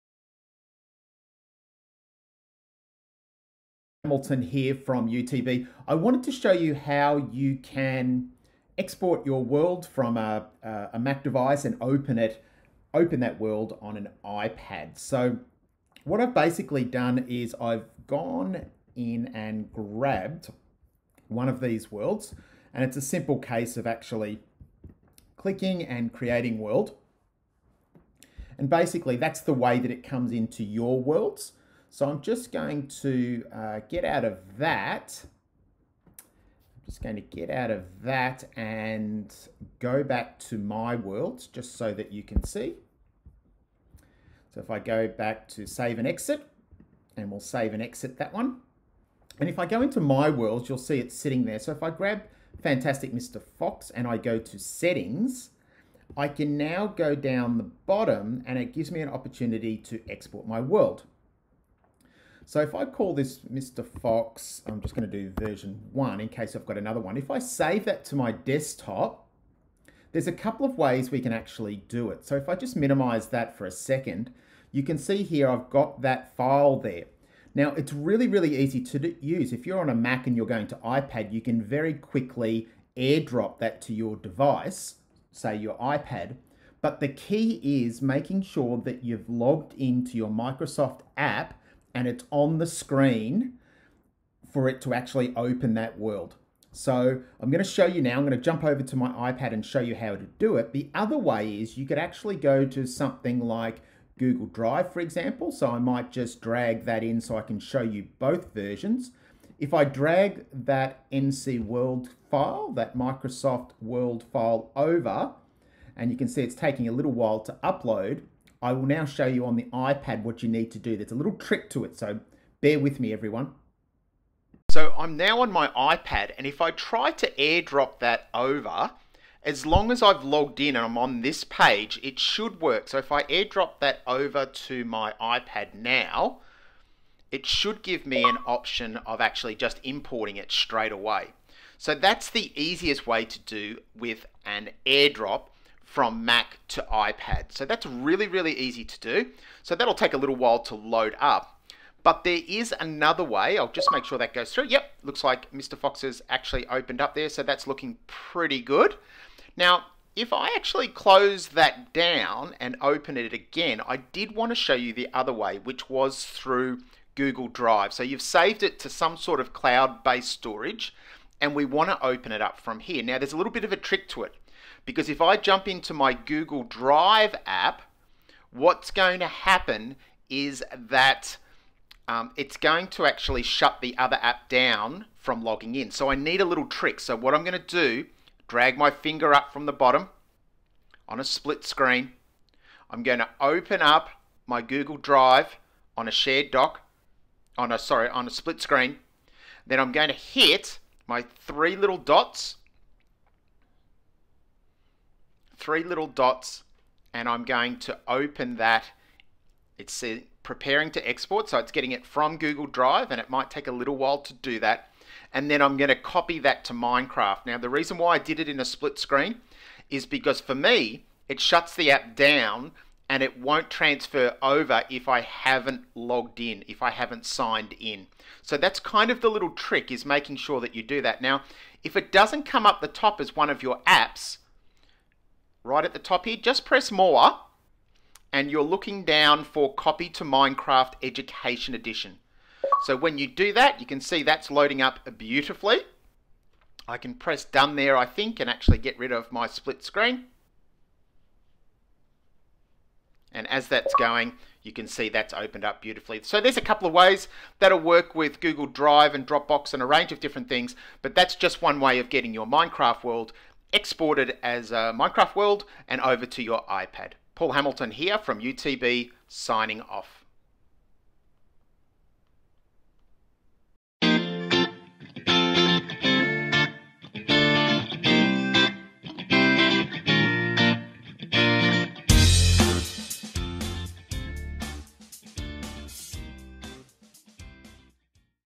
Importing and exporting Minecraft worlds from Mac to iPad
Paul Hamilton from Using Technology Better takes you through transferring worlds from Mac to iPad.